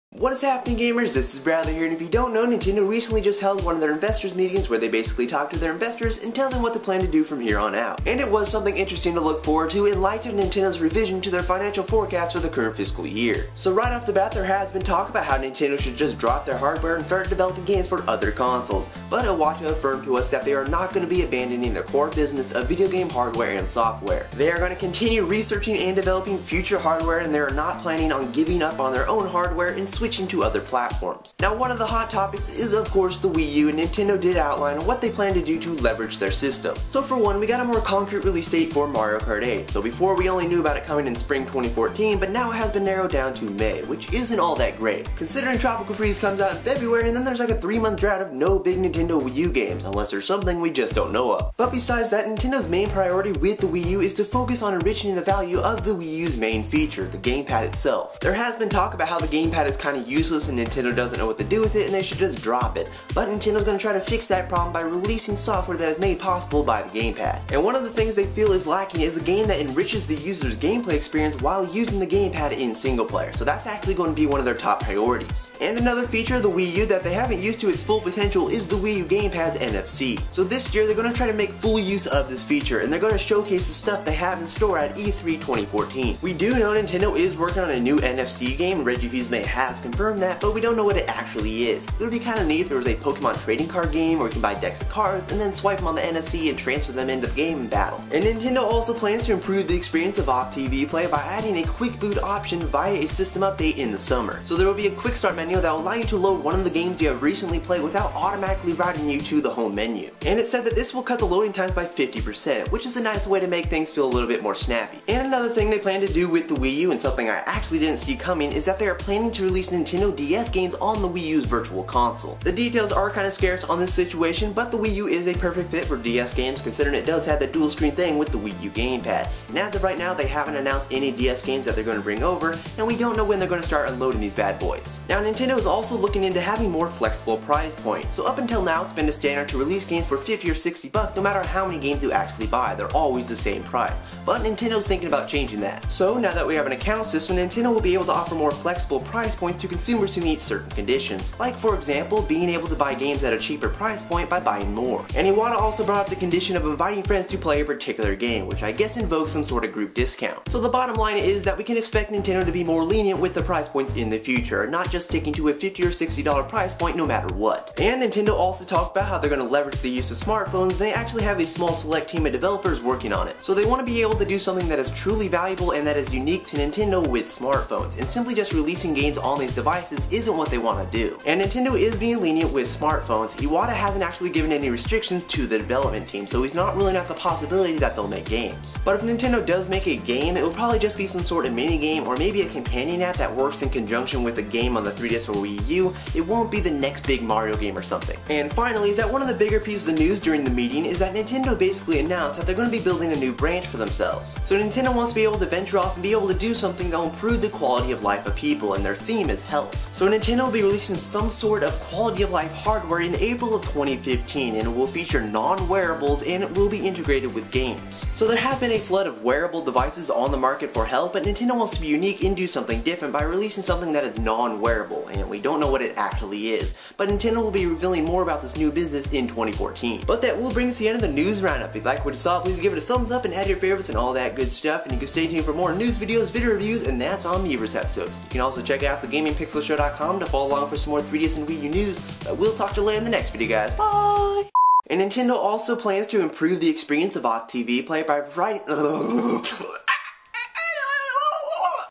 Mario Kart 8 in May, NFC at E3, DS Games on Wii U VC, New "QOL" Hardware
Follow for Nintendo 3DS and Wii U News, Rumors, and Reviews!

Iwata Affirms That Video Game Hardware & Software Will Remain Their Core Business

Iwata Talks About Getting the Wii U Back on Track, Priority is Focusing on GamePad Usage

Iwata Talks About Bringing Nintendo DS Games to the Wii U VC

Iwata Talks About the Quick Start Menu With the Wii U GamePad

Iwata Talks About Making Use of the GamePad's NFC, Will Showcase NFC at E3 2014

Iwata Talks About Leveraging Smart Devices, Open to Making Games on Smartphones